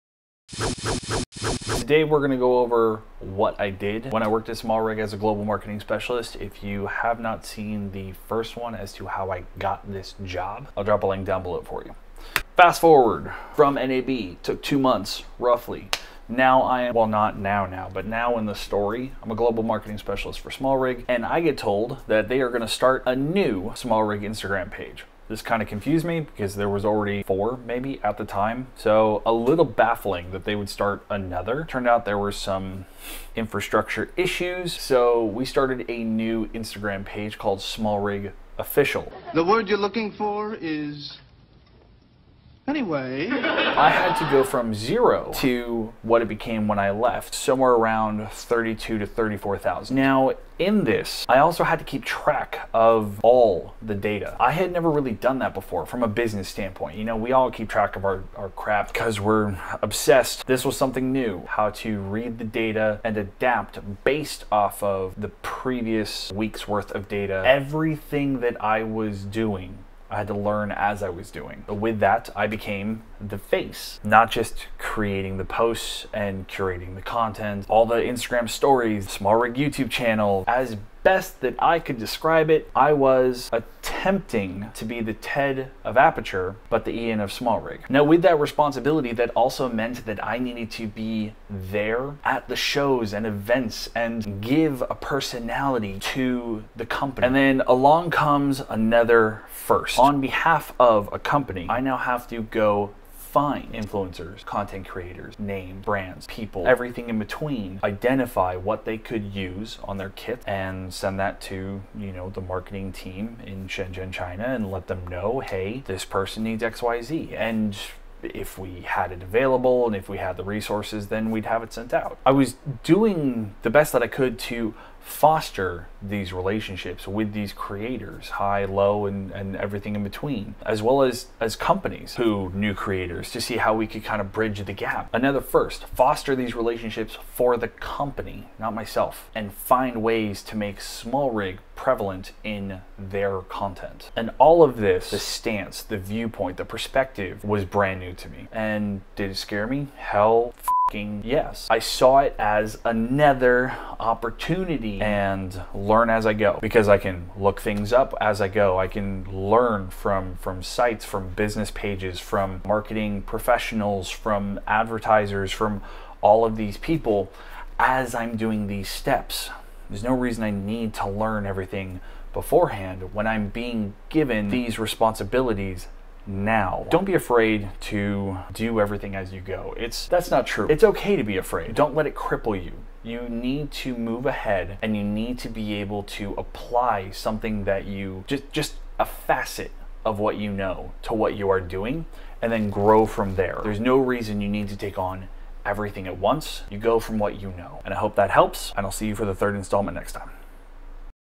What Did I Do Working at SmallRig as a Global Marketing Specialist?
Key Light
Wall Mount
Accent Light
A Camera
B Camera
External Monitor